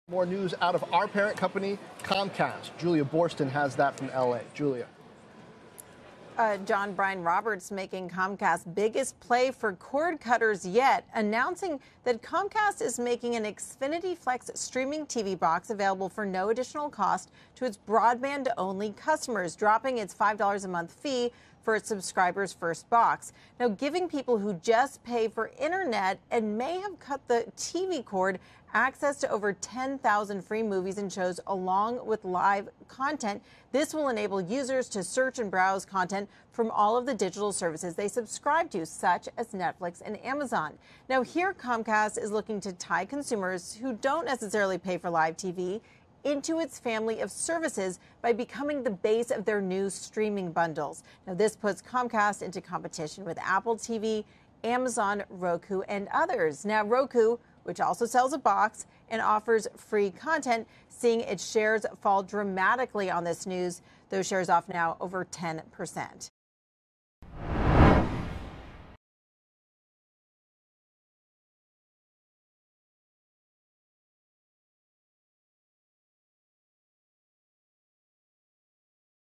Comcast makes streaming TV box free for internet customers
Comcast, CNBC's parent company, announced that it will bundle its streaming TV box with its internet-only service. CNBC's Julia Boorstin reports.